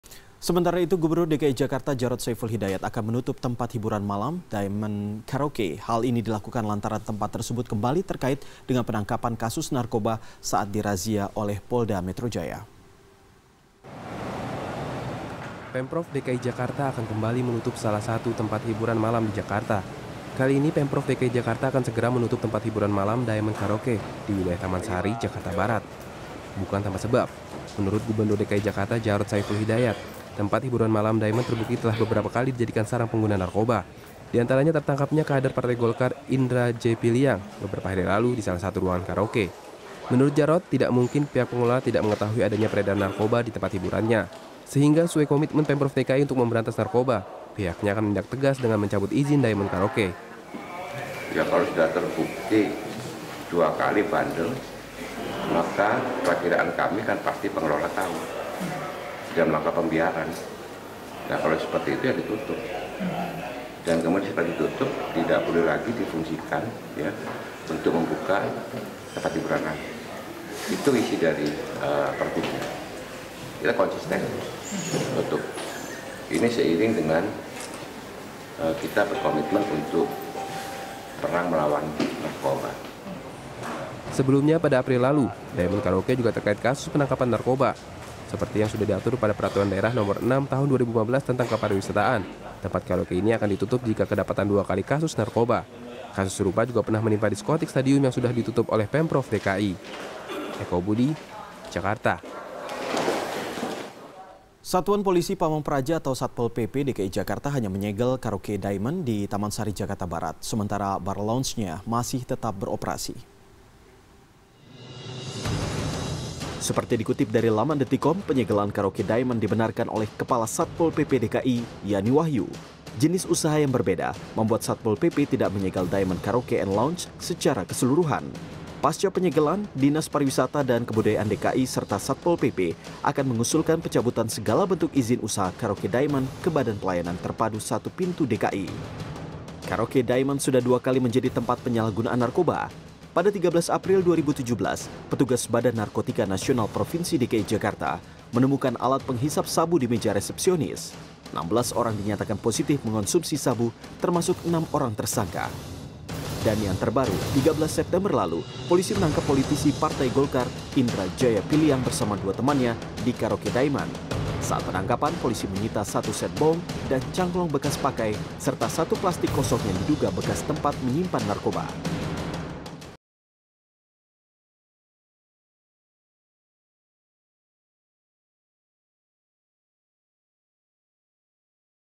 Tempat Hiburan Malam Diamond Karaoke Akan Ditutup!
Gubernur DKI Jakarta, Djarot Saiful Hidayat akan menutup tempat hiburan malam, Diamond Karaoke

Hal ini dilakukan lantaran tempat tersebut kembali terkait dengan penangkapan kasus narkoba, saat dirazia oleh Polda Metro Jaya

Like & Follow Facebook:
CNN Indonesia

cnnindonesia